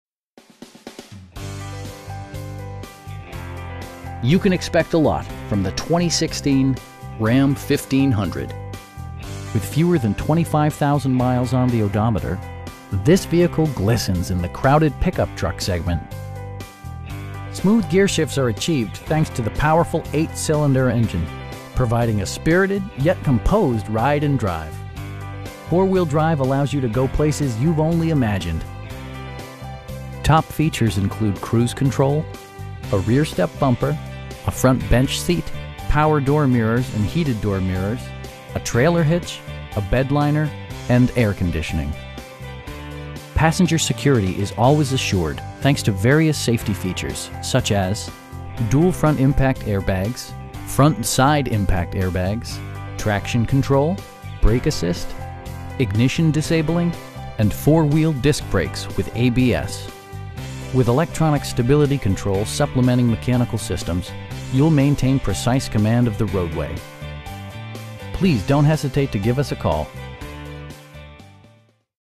2016 Ram 1500 Express in Longwood, FL 32750
Orlando Kia North
625 N US HWY 17-92

You can expect a lot from the 2016 Ram 1500 With fewer than 25,000 miles on the odometer, this vehicle glistens in the crowded pickup truck segment! Smooth gearshifts are achieved thanks to the powerful 8 cylinder engine, providing a spirited, yet composed ride and drive. Four wheel drive allows you to go places you've only imagined. Top features include cruise control, a rear step bumper, a front bench seat, power door mirrors and heated door mirrors, power windows, a trailer hitch, a bedliner, and air conditioning. Passenger security is always assured thanks to various safety features, such as: dual front impact airbags, front side impact airbags, traction control, brake assist, ignition disabling, and 4 wheel disc brakes with ABS. With electronic stability control supplementing mechanical systems, you'll maintain precise command of the roadway. We pride ourselves in consistently exceeding our customer's expectations. Please don't hesitate to give us a call.